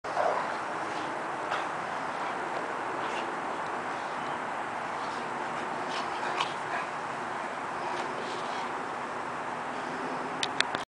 Boxer mixes play in the snow
Wrestling dogs in the snow. Boxer/pit mix and boxer/ridgeback mix.